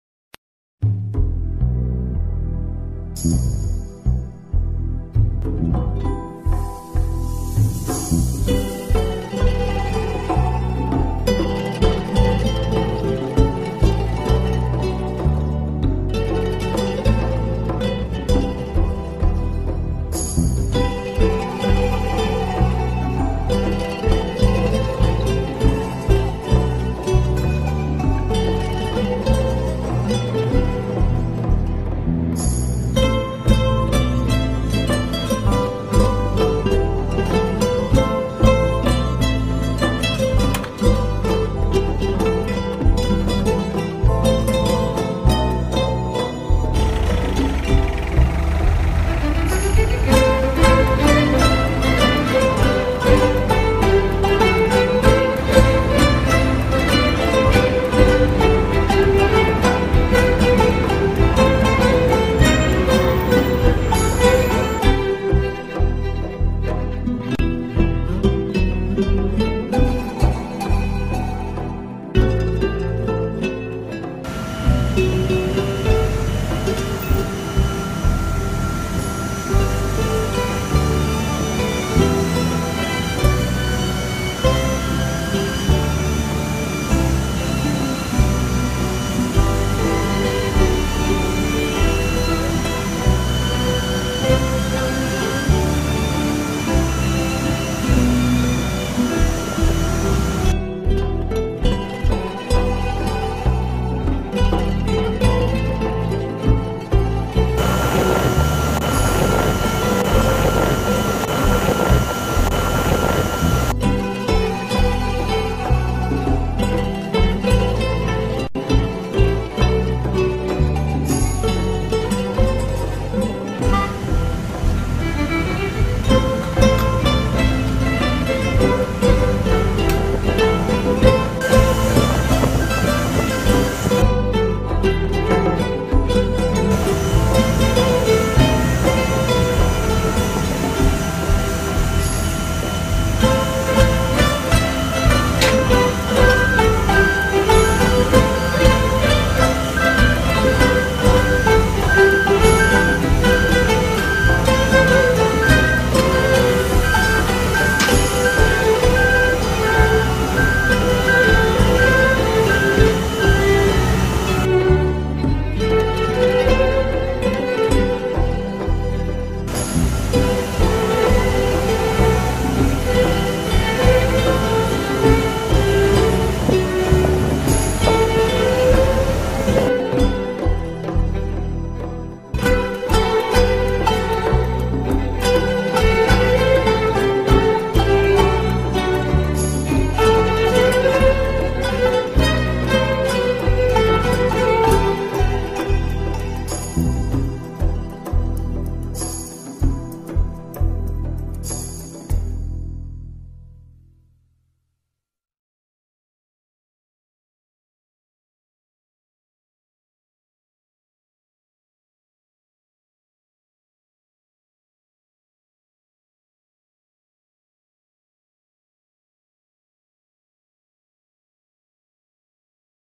Greece ATH SAFCO (Training Material)
This video is ONLY for training purposes.
SAFCO is the into-plane handler at Athens International Airport "El. Venizelos"